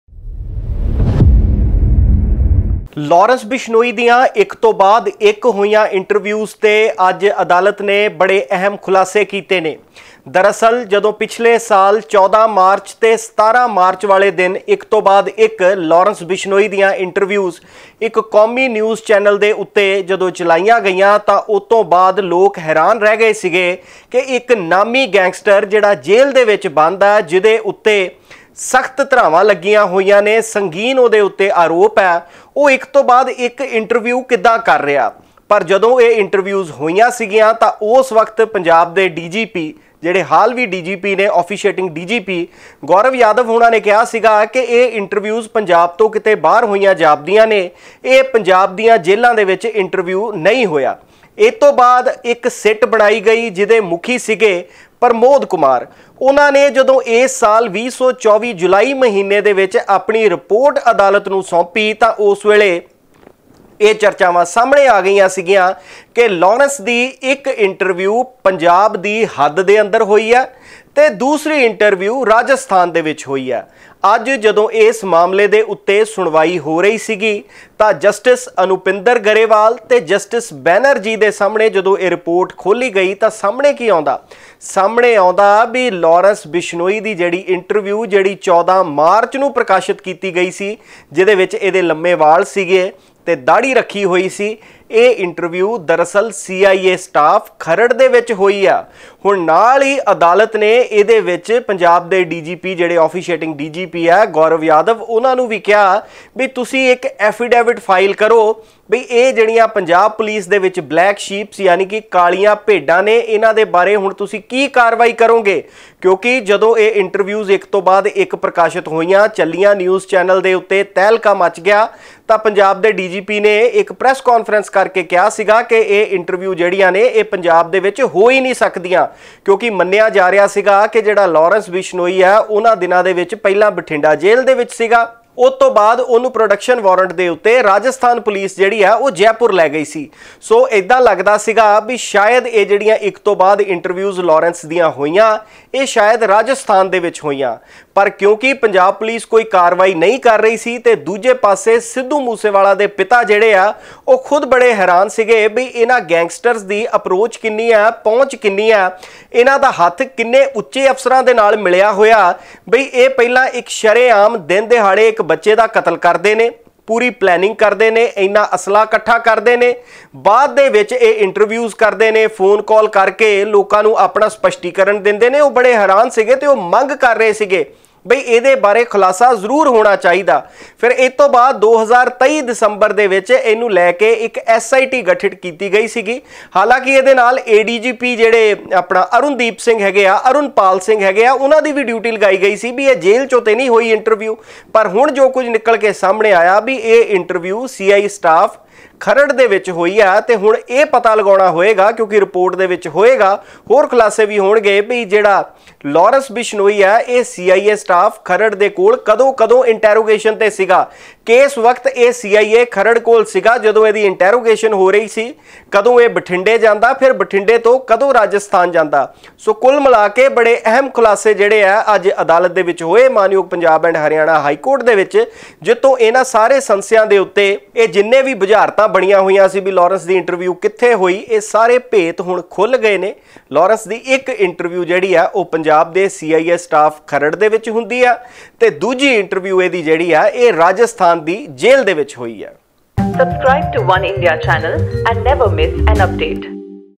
ਪੰਜਾਬ ਦੇ ਇਸ ਥਾਣੇ 'ਚ ਹੋਈ ਲਾਰੈਂਸ ਦੀ ਇੰਟਰਵਿਊ ਹਾਈ ਕੋਰਟ 'ਚ ਹੋਇਆ ਖ਼ੁਲਾਸਾ, ਭਾਜੜਾਂ ਪੁਲਿਸ ਨੂੰ...
ਪੰਜਾਬ ਦੇ ਇਸ ਥਾਣੇ 'ਚ ਹੋਈ ਲਾਰੈਂਸ ਦੀ ਇੰਟਰਵਿਊ
ਹਾਈ ਕੋਰਟ 'ਚ ਹੋਇਆ ਖ਼ੁਲਾਸਾ,  ਭਾਜੜਾਂ ਪੁਲਿਸ ਨੂੰ